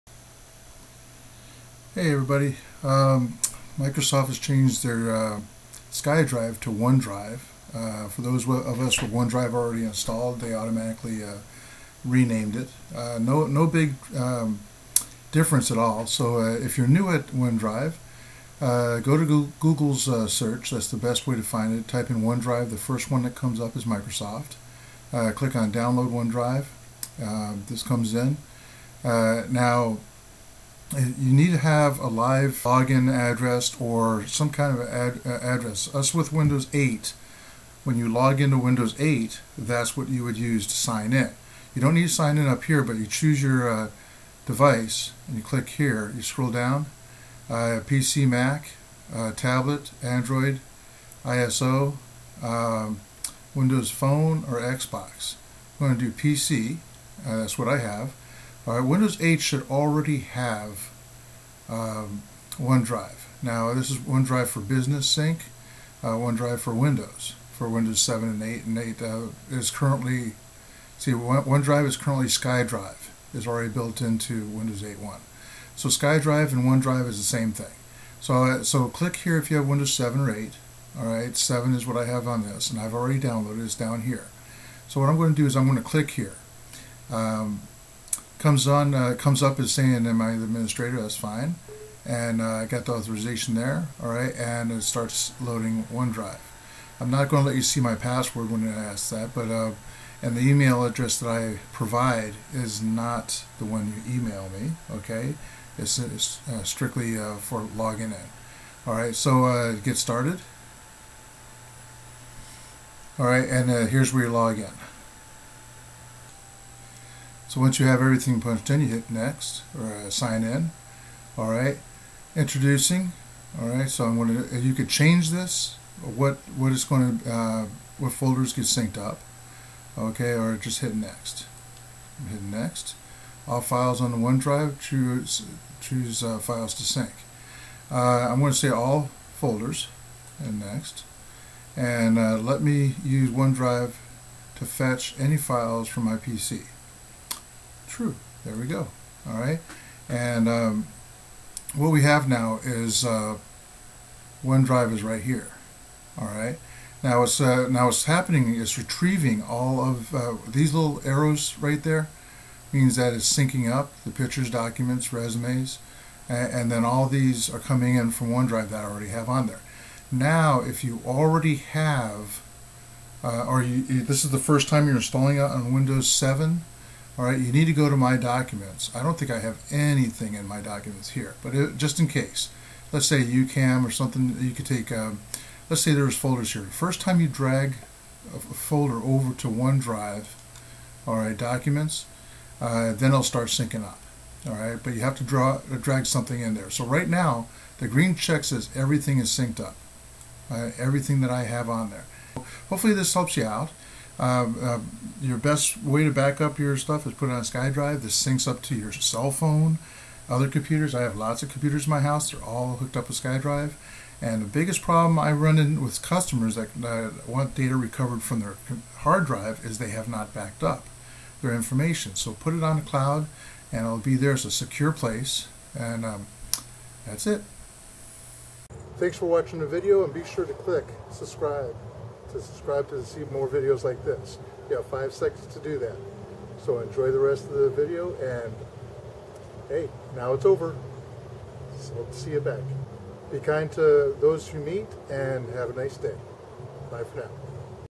How to Install and Use OneDrive
How to Install MicroSoft OneDrive
The use is also covered in this video.